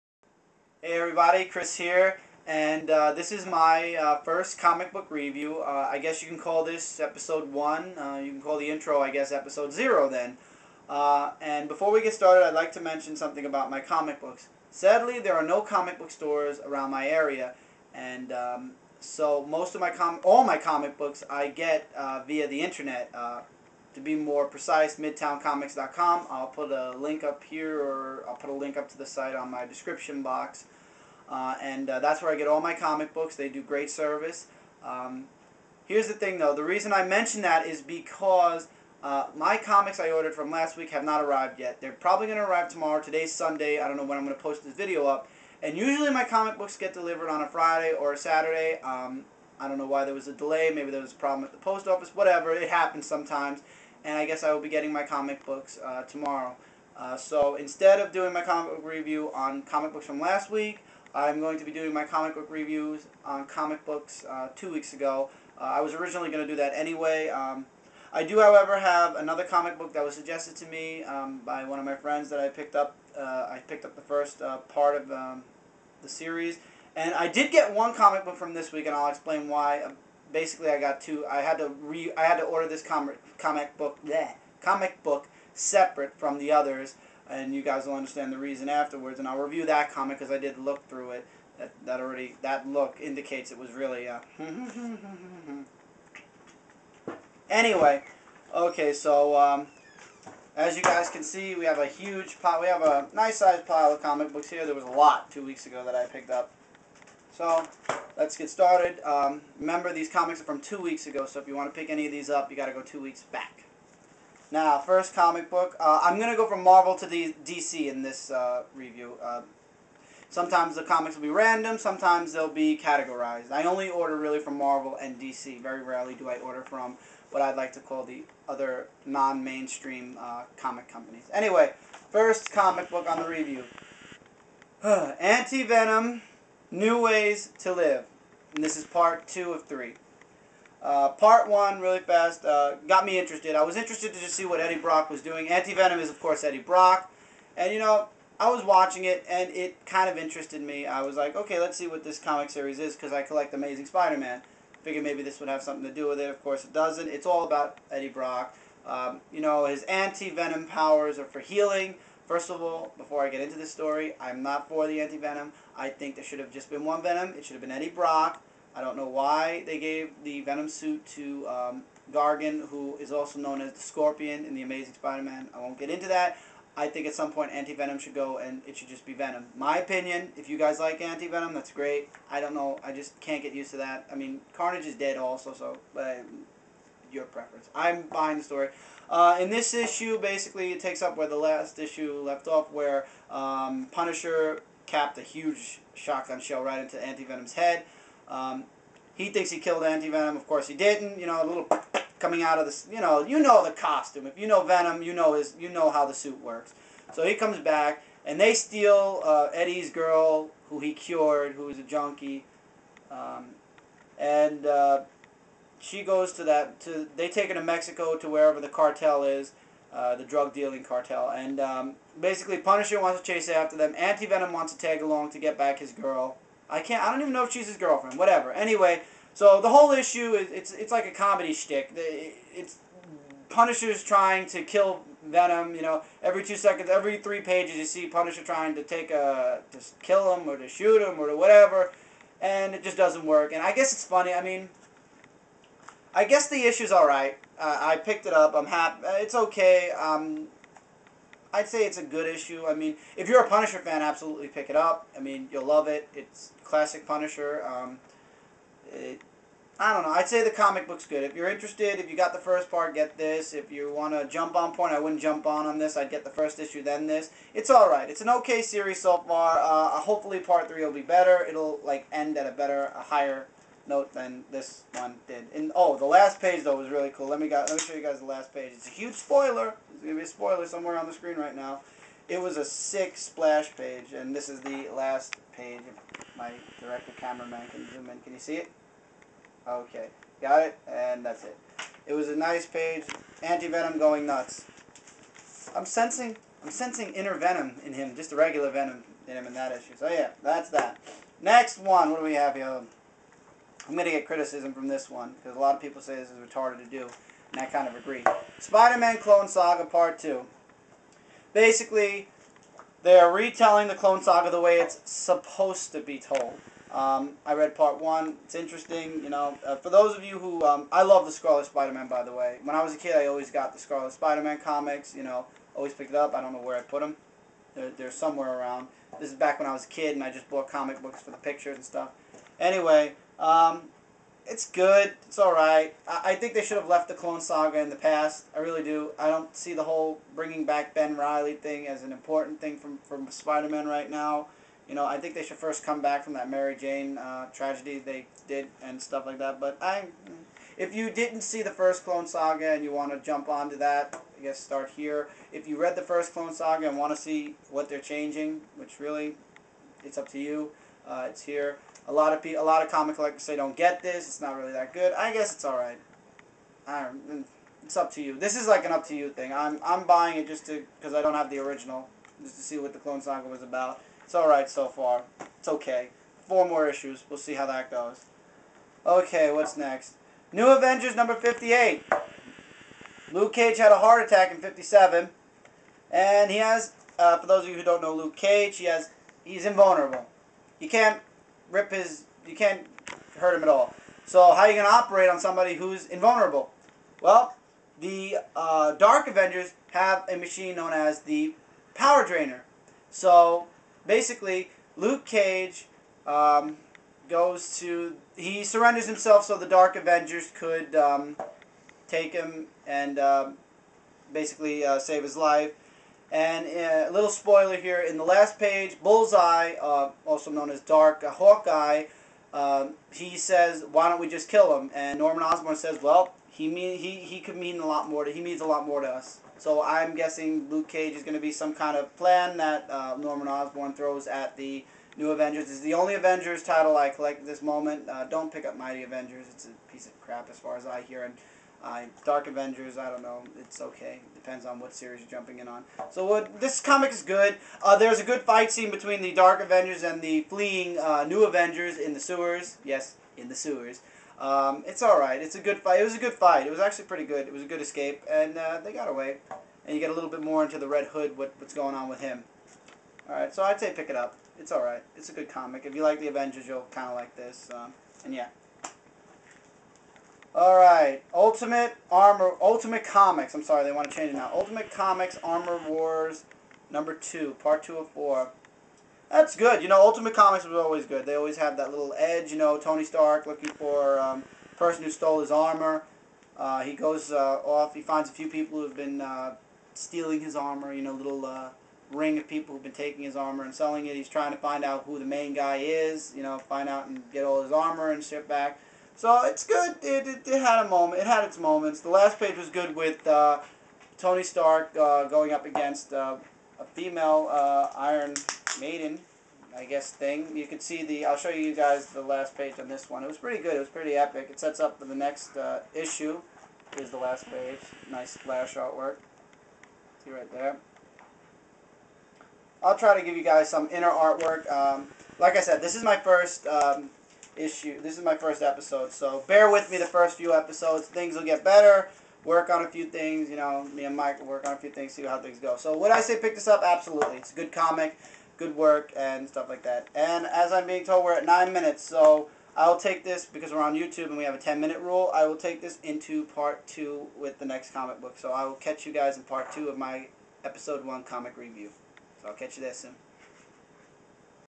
Comic Book Review: Episode 1 - Part 1
My first comic review episode. All comics I get are via Midtown Comics (internet), this episodes is of comics I got from 2 weeks ago (10/28/09) instead of last weeks because I have yet to receive them (those will be in the next episode I will make sometime this week). After a brief intro, the comics I talk about in part one are: Anti-Venom: New Ways to live #2 of 3, Spider-Man: Clone Saga #2 of 6, New Avengers #58, and Ultimate Comics: Armor Wars #2 of 4.